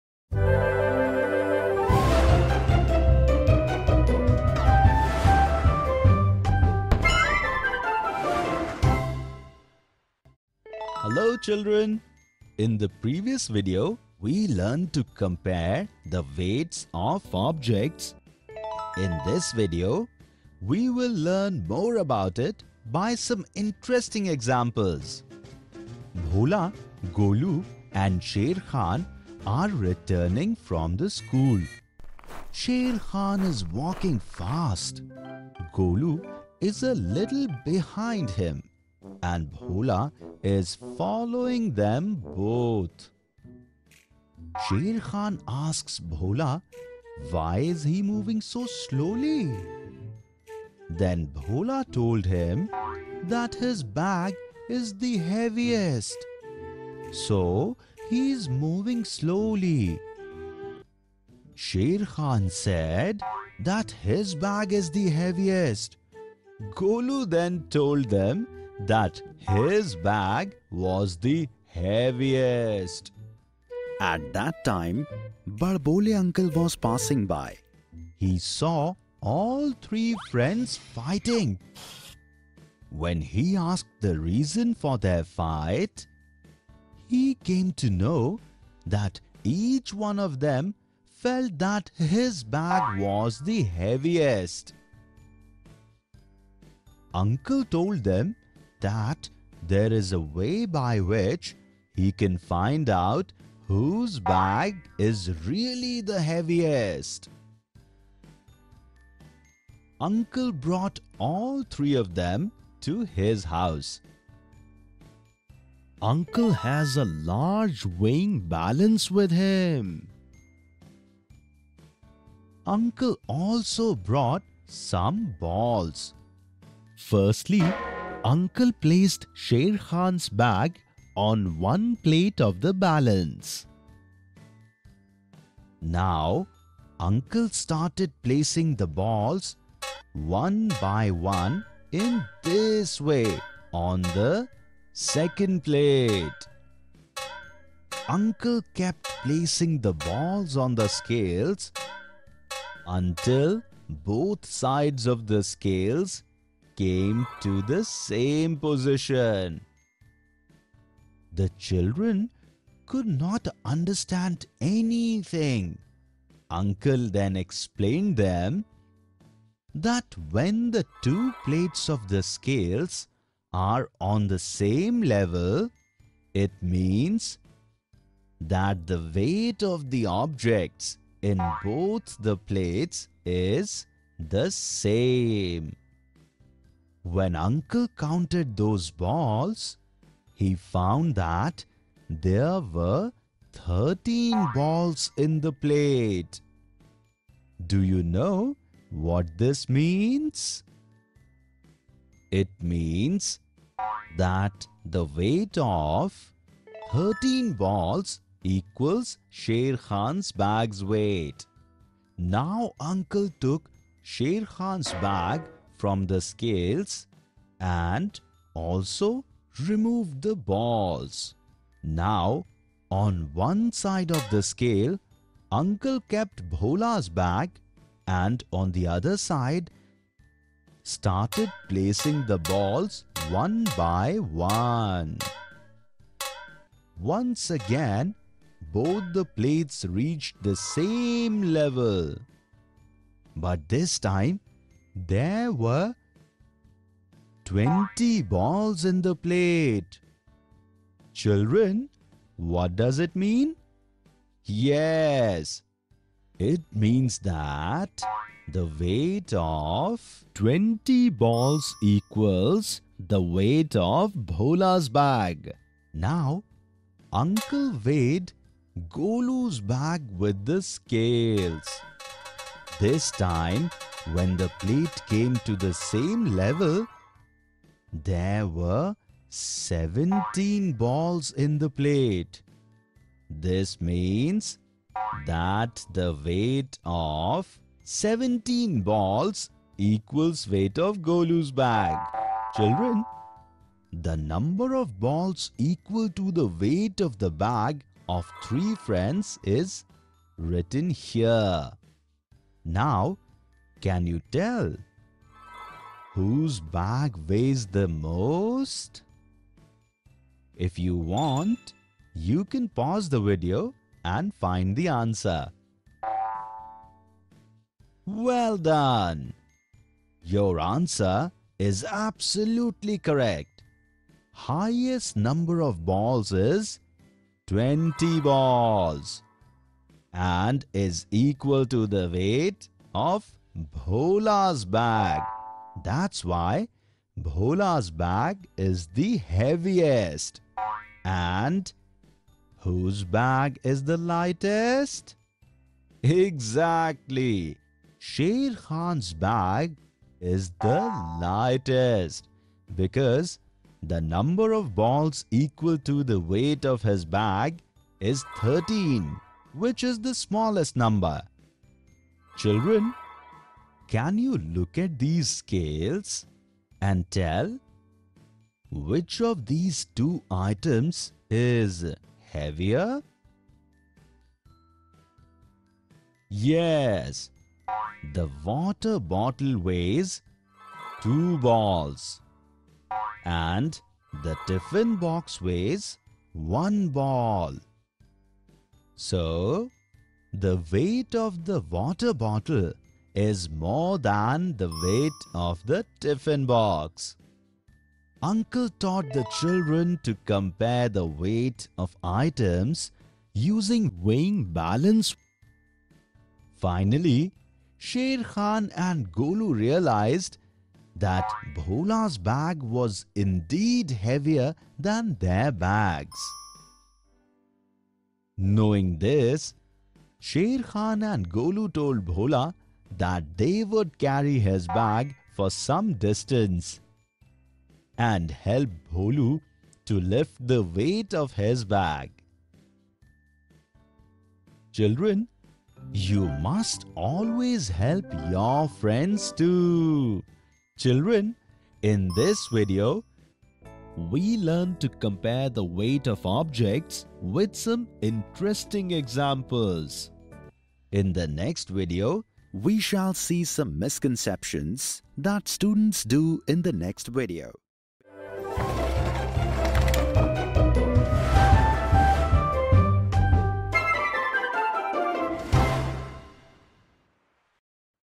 Estimation of weight (non standard units) | Part 2/3 | English | Class 3
Estimation of weight (non standard units) | Part 2/3 | English | Class 3 |  | NCERT |

Estimation of weight (non standard units) | Measurement of Weight | Grade 3 | NCERT | Who is heavier

Welcome to part 2 of Estimation of weight (non standard units). In this video, we will discuss various examples of Estimation of weight (non standard units).